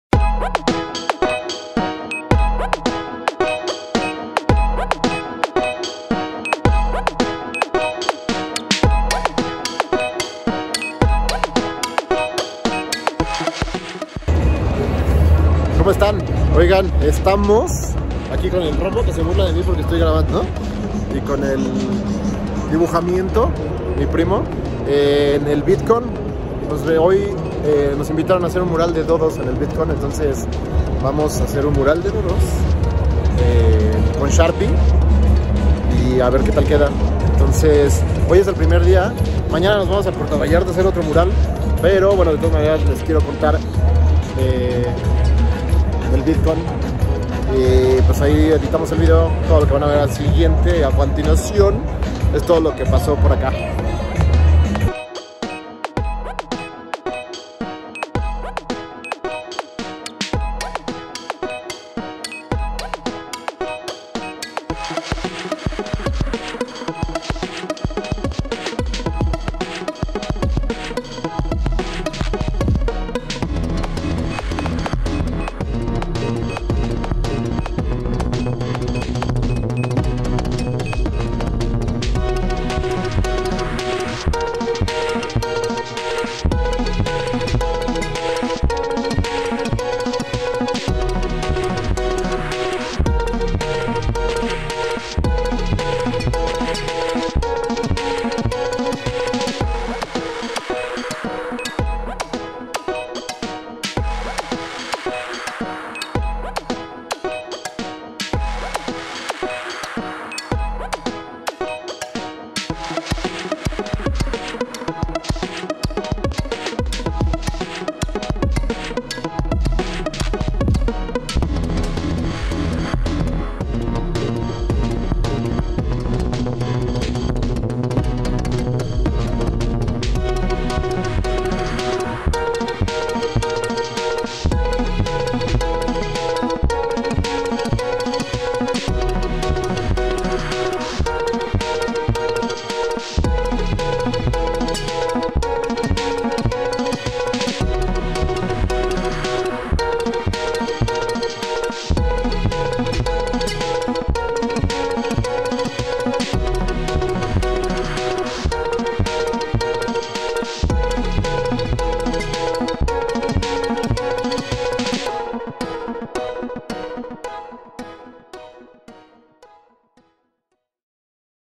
LLENAMOS DE DOODLES EL VIDCON MÉXICO 2022
Acá les dejo el proceso del mural que hicimos junto con @Sharpie_Latinoamerica en el  @VidCon
Nos invitaron a ir al stand de Sharpie para llenar un mural con Dodos en uno de los eventos más importantes para creadores de contenido e influencers. Así que nos lanzamos al Vidcon y la verdad es que estuvo super cool, había muchísimos stands de diferentes plataformas y marcas, además de pláticas y conferencias super interesantes. Ya ahí, decidimos poner el corachón al centro diciendo “I ❤️ Sharpie” y al rededor los Dodos, a algunos de ellos los pusimos diciendo nombres de las plataformas digitales que estuvieron presentes como @tiktok @spotifymexico @Meta @pinterestlatam @youtubelatam y además los nombres de algunos de los artistas y creadores presentes en el Vidcon como  @Paco De Miguel    @Aczino Rapsoul    @Pongamoslo a prueba    @Juanpa Zurita    @Mario Bautista    @SKabeche    @Luisito Comunica    @Robegrill   @La Ruta de la Garnacha    @Leyendas Legendarias   @troop y más. (Etiqueten a sus favoritos en los comentarios)😬
La neta es que nos la pasamos super chido y mientras estuvimos haciendo el mural, muchos de ustedes pasaron a saludar, así muchísimas gracias a los que se dieron una vuelta. ❤️‍🔥❤️‍🔥❤️‍🔥❤️‍🔥
Gracias también a @sharpie_latinoamerica y a @sbtalento
Igual muchas gracias a @eldibujamiento y a @romboou que estuvieron ayudándome.
Esperemos pronto volver a hacer uno donde puedan ir a verlo. 😊❤️‍🔥
Si te prende❤️
Que incendie❤️‍🔥